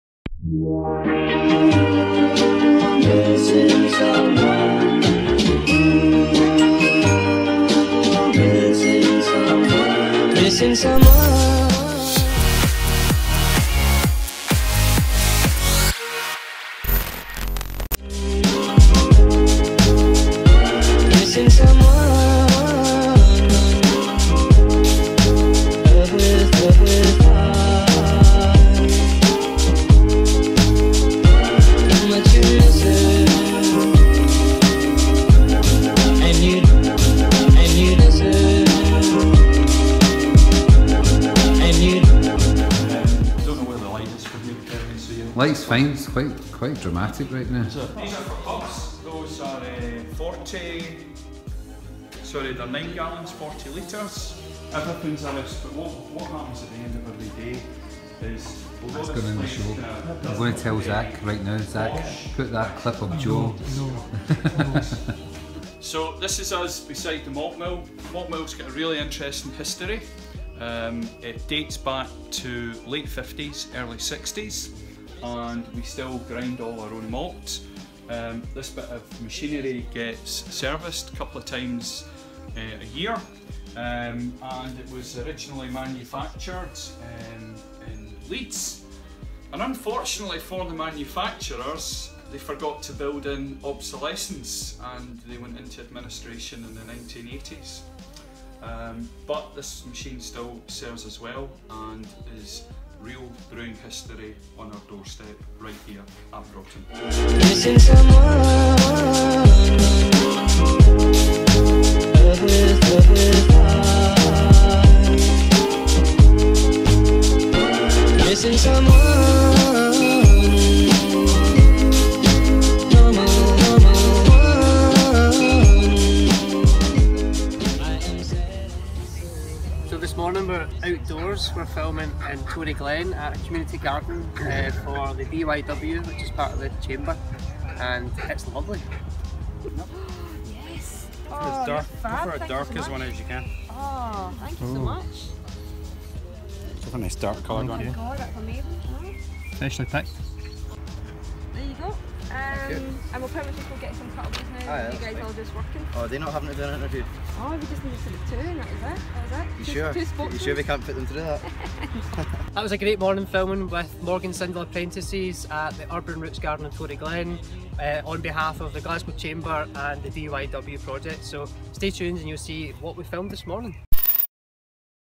29ERS Show - EPISODE 67
That's right. Episode 67 of the 29ERS Show was shot on a mobile phone. Completely. Would you have been able to tell if we didn't tell you?
In this episode we follow Kev on his Broughton Brewery adventure and our amazing production team takes us behind the scenes of a shoot for the DYW Glasgow, proudly supported by the Glasgow Chamber of Commerce.